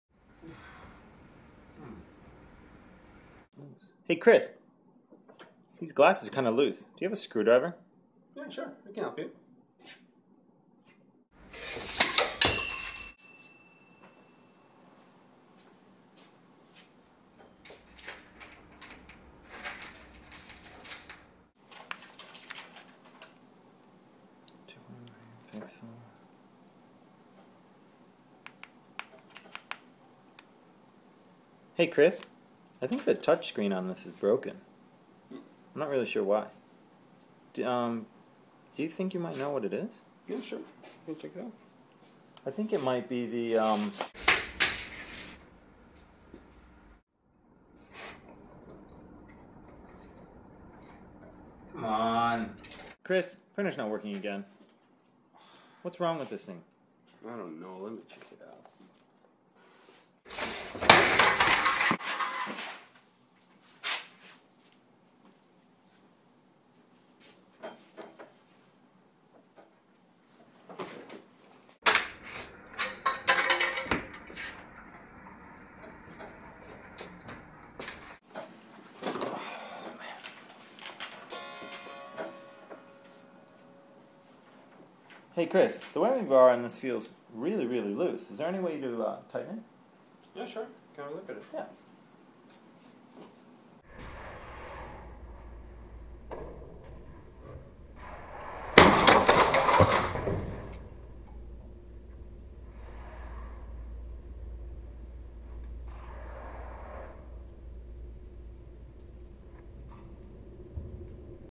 Mr Fix It
Kris offers a new approach to fixing things at the office.